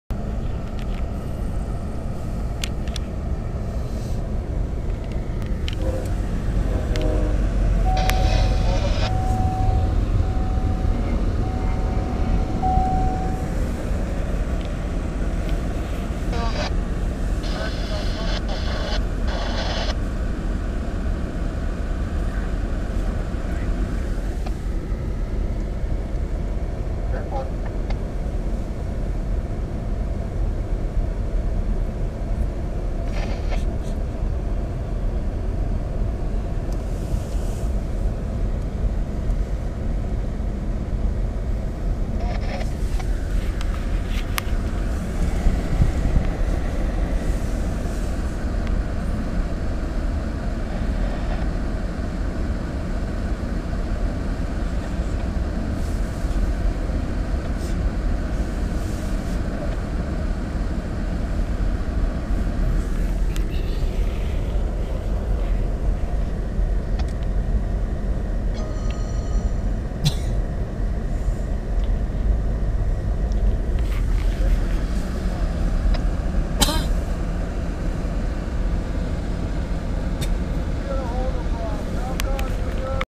CSX Train In The Snow Elevated Tracks 1/13/12
Train CSX Train In The Snow Elevated Tracks 1/13/12 Big Long and Massive Trains Here at Wide World of Trains. I want to start doing Airplane and Jet videos at Air Shows Some Day